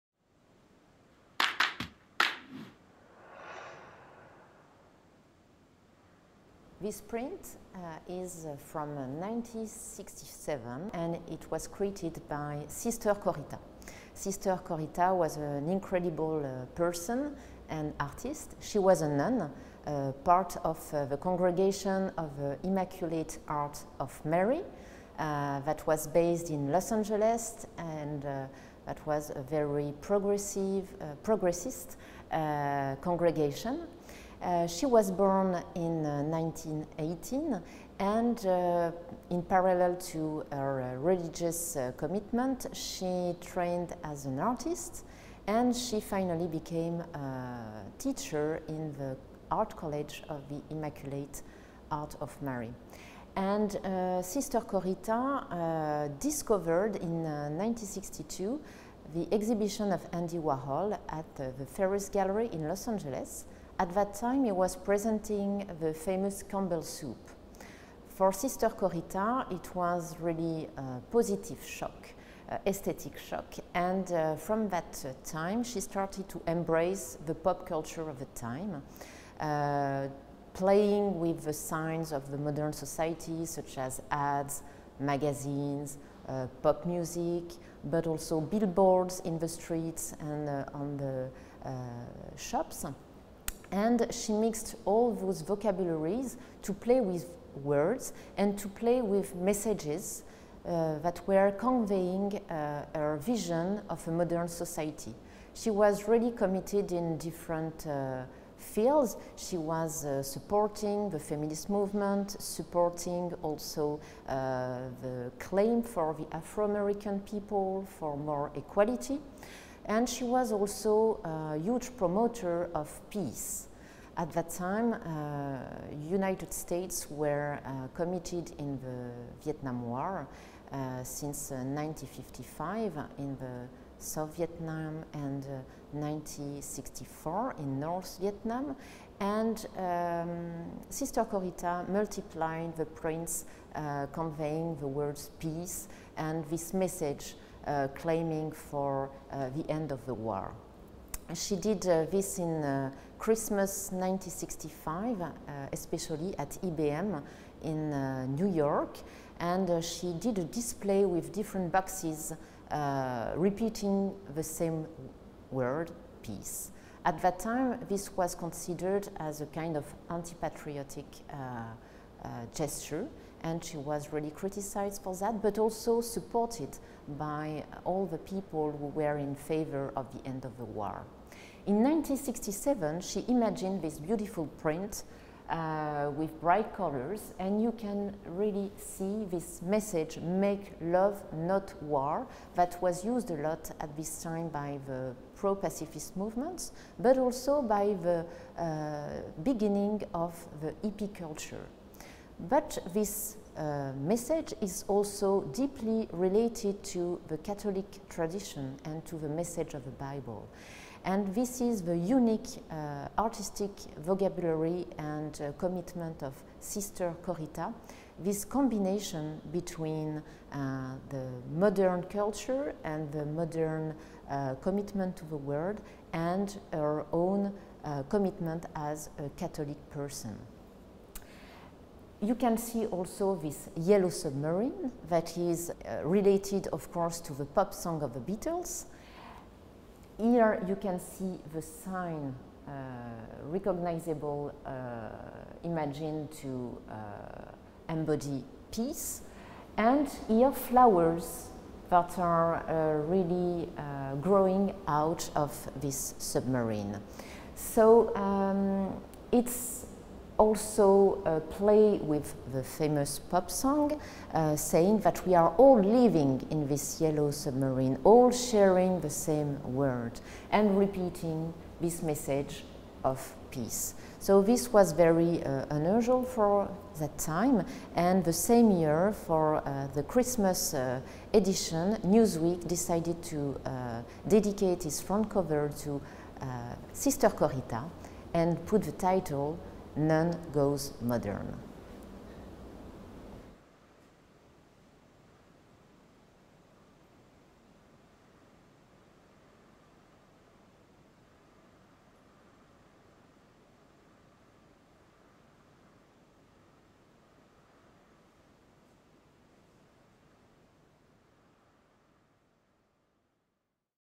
MAMAC | I VISIT: "One minute with an artwork" Episode 5
3 minutes with Hélène Guenin, Lélia Decourt, Rebecca Francois and Olivier Bergesi to discover artworks from the museum's collection - Ben Walter montage from the association le Cercle rouge.